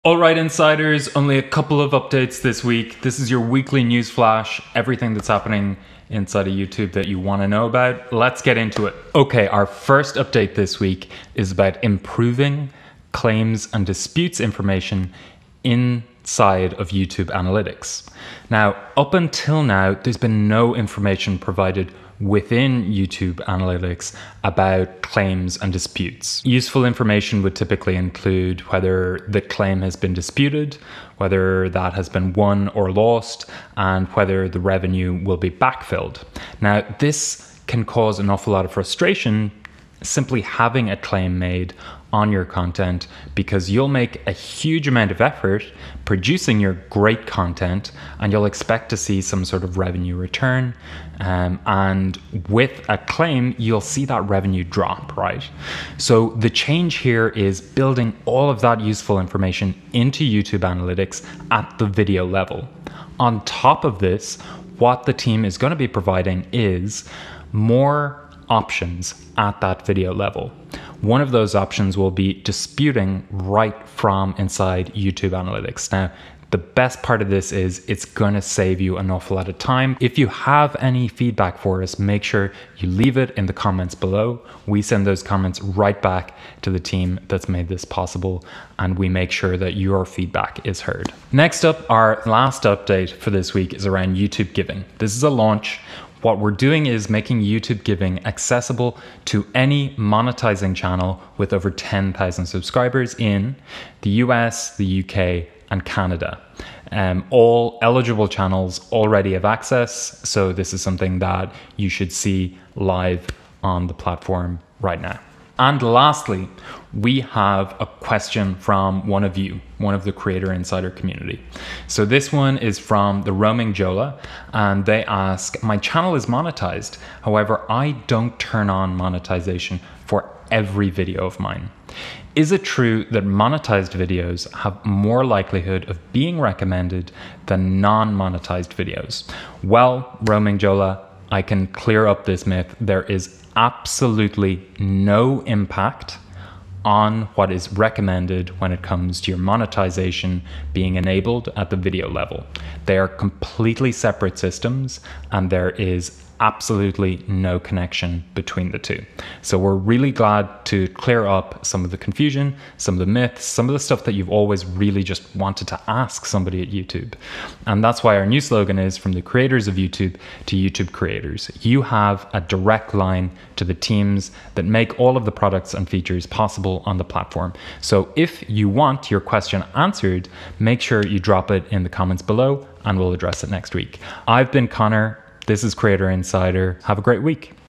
Improved Claims & Disputes Experience in YTA, YT Giving Expansion, and YOUR Questions!
Hello Insiders! Short update this week.

Chapters:

0:00 Intro
0:09 Improved Claims & Disputes Experience in YTA
1:31 [Launch] YT Giving Expands to 10K+ Subs in US, UK, & CA
1:55 Trivia Winner @TheRoamingJhola

👕👚SWAG 🎽☕

100% of the proceeds automatically support Red Cross